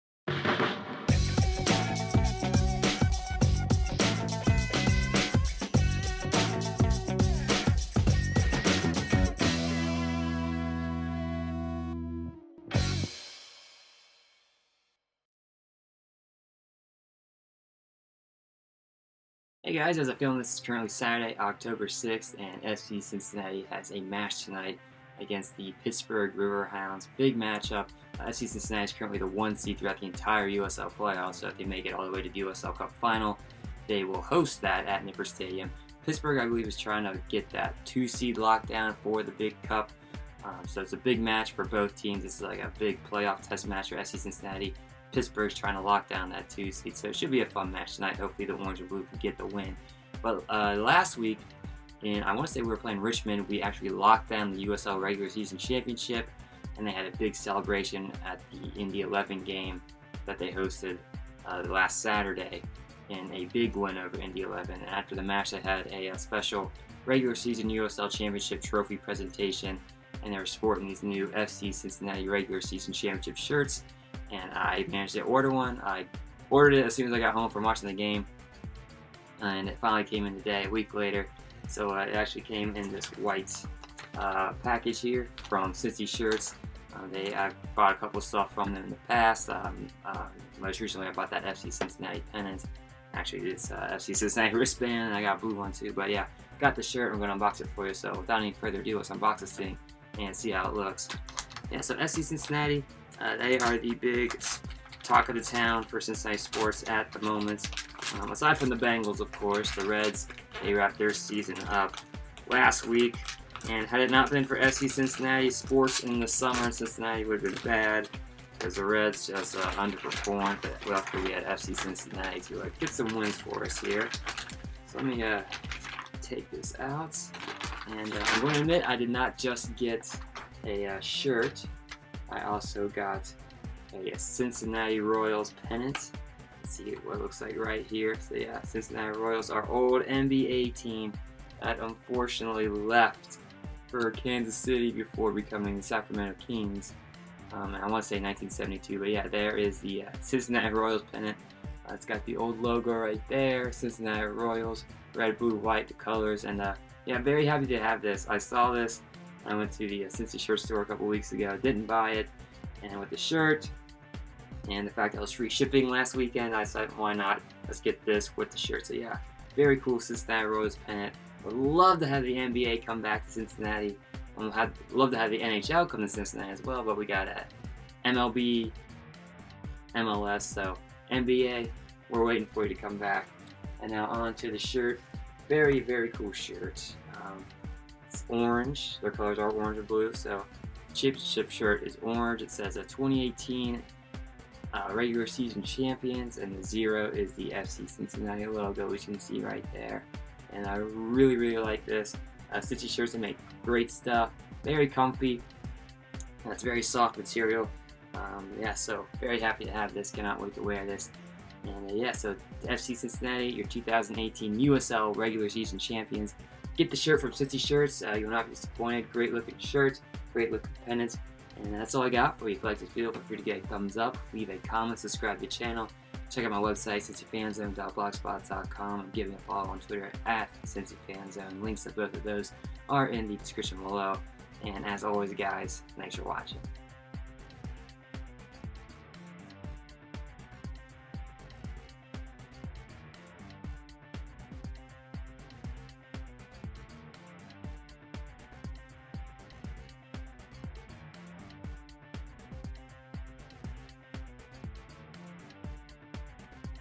FC Cincinnati USL Regular Season Championship Shirt Unboxing
Ryan of Cincy Fan Zone unboxes an FC Cincinnati USL Regular Season Championship shirt as well as a Cincinnati Royals pennant.